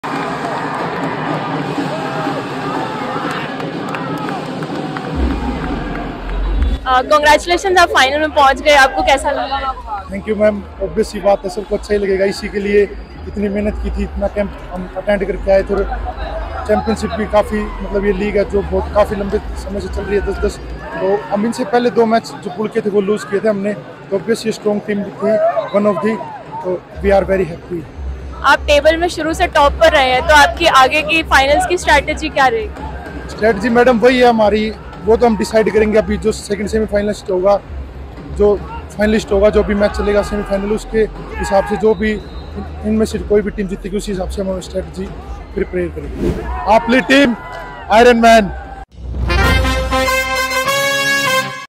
Maharashtra Ironmen reach the final | Premier Handball League
The Maharashtra Ironmen put on a dominating show against the home team, Rajasthan Patriots, defeating them by a 10 goal margin and became the first team to enter the final of the inaugural edition of the Premier handball League.